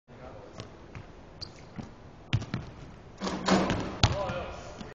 Cartwheel Dunk
Scott Rogers, a Trine University Basketball Player, cartwheels to a straight dunk by the rim